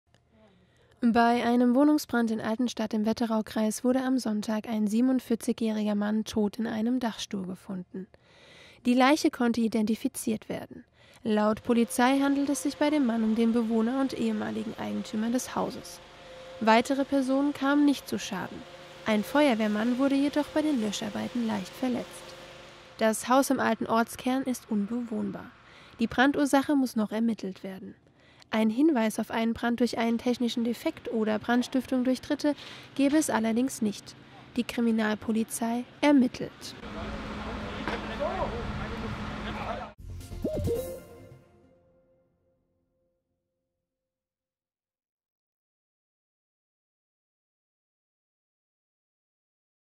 Dachstuhl in Altenstadt komplett ausgebrannt | hessenschau
Bei dem am Sonntag bei einem Dachstuhlbrand in Altenstadt (Wetteraukreis) entdeckten Toten handelt es sich um den Bewohner des ausgebrannten Hauses. Den Leichnam des 47-Jährigen hätten Feuerwehrleute bei den Löscharbeiten im Dachgeschoss gefunden, berichtete die Polizei am Montag. Das Haus im Ortskern ist nach dem Brand nicht bewohnbar.

Die genaue Todesursache ist laut Polizei noch unklar. Wie es zu dem Brand kommen konnte, muss auch noch geklärt werden. Einen Hinweis auf einen Brand durch einen technischen Defekt oder Brandstiftung durch Dritte gebe es allerdings nicht, teilte die Polizei mit. Die Kriminalpolizei ermittelt. Bei den Löscharbeiten wurde ein Feuerwehrmann am Fuß verletzt.